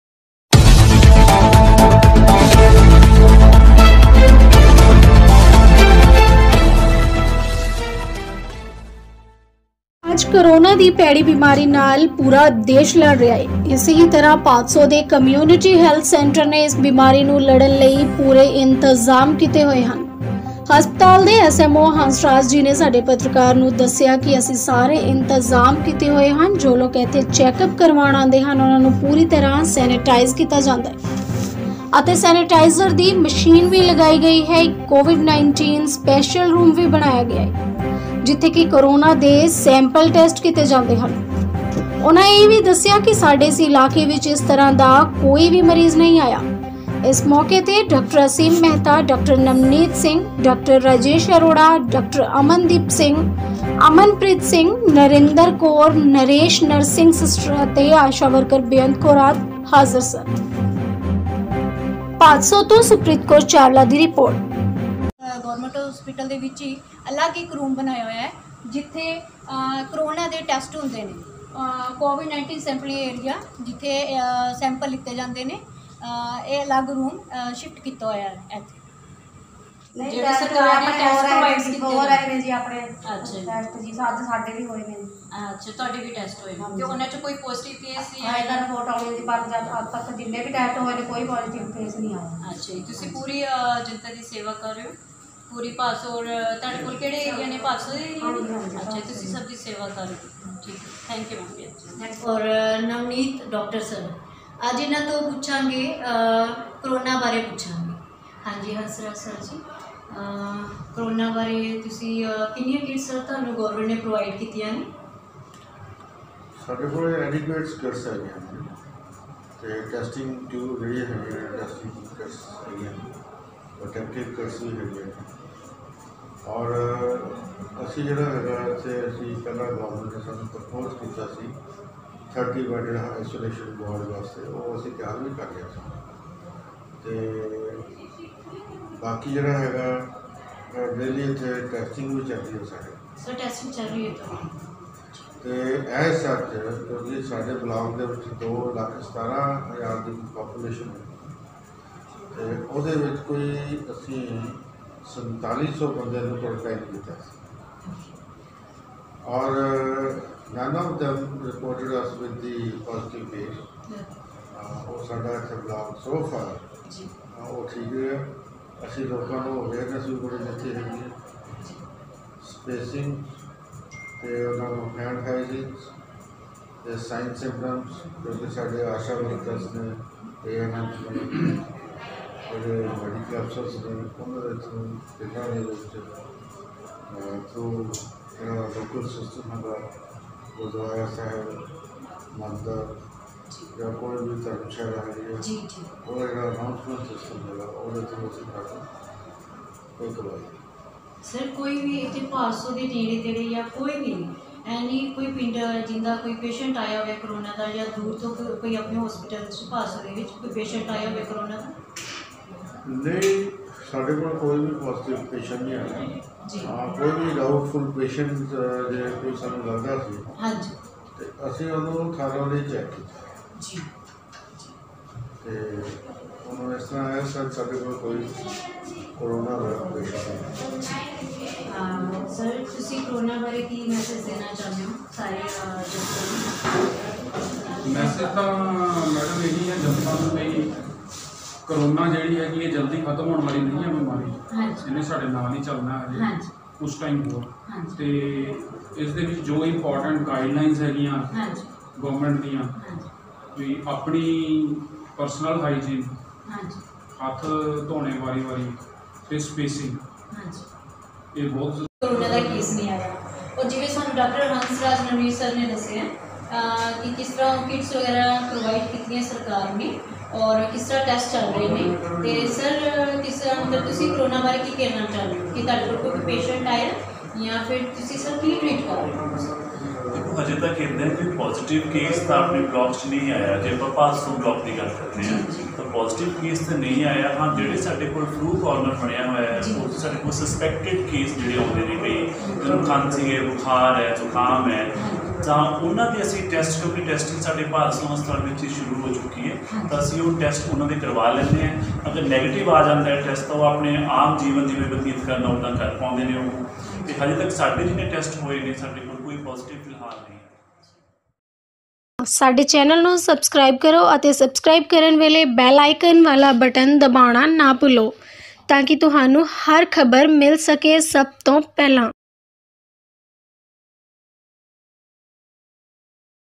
ਭਾਦਸੋਂ ਦੇ ਕਮਿਊਨਿਟੀ ਹੈਲਥ ਸੈਂਟਰ ਵਿਚ ਕੋਵਿਡ 19 ਸਪੈਸ਼ਲ ਰੂਮ ਹੈ ਅਤੇ ਇਲਾਕੇ ਵਿਚ ਇਸ ਤਰ੍ਹਾਂ ਦਾ ਕੋਈ ਵੀ ਮਰੀਜ਼ ਨਹੀਂ
ਭਾਦਸੋਂ ਦੇ ਕਮਿਊਨਿਟੀ ਹੈਲਥ ਸੈਂਟਰ ਵਿਚ ਕੋਵਿਡ 19 ਸਪੈਸ਼ਲ ਰੂਮ ਹੈ ਅਤੇ ਇਲਾਕੇ ਵਿਚ ਇਸ ਤਰ੍ਹਾਂ ਦਾ ਕੋਈ ਵੀ ਮਰੀਜ਼ ਨਹੀਂ ਆਇਆ: ਐਸਐਮਓ ਹੰਸ ਰਾਜ

Rep: Supreet Kaur Chawla